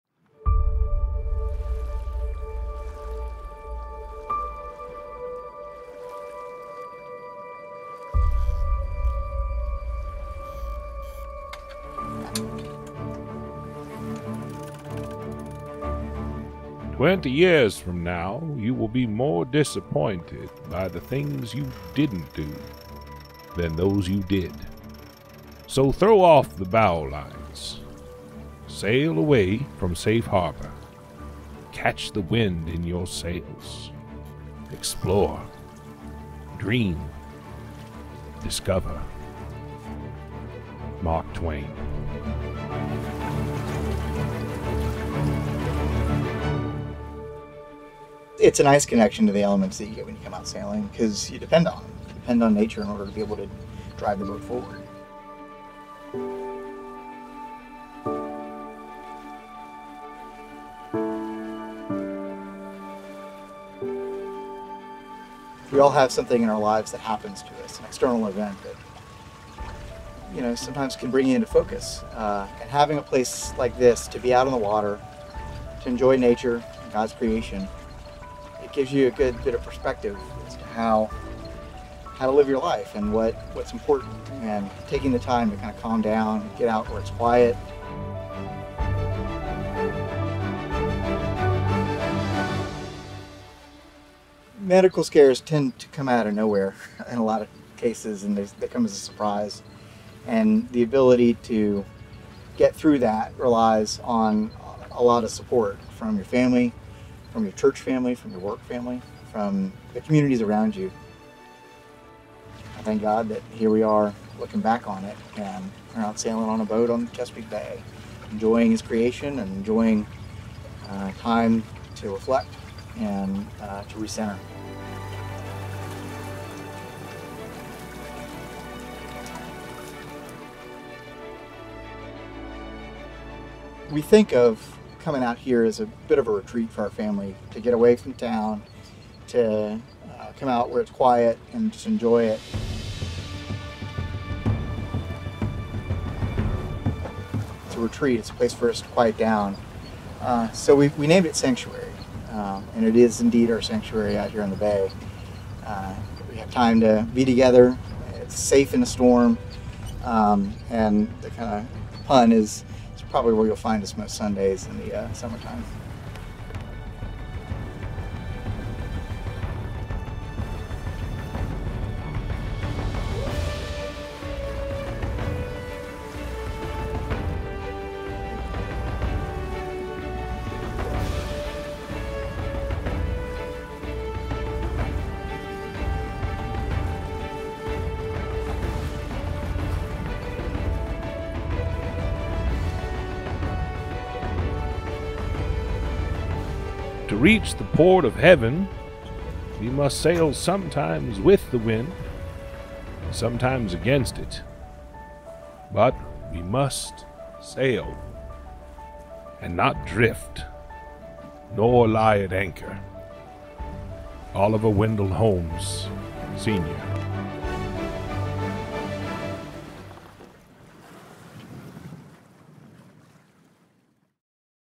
Bailey Edwards on 'Sanctuary'
GPC member Bailey Edwards and his family on finding their Sanctuary on the sea. Included in the live-stream worship service on Sunday, August 17, 2020.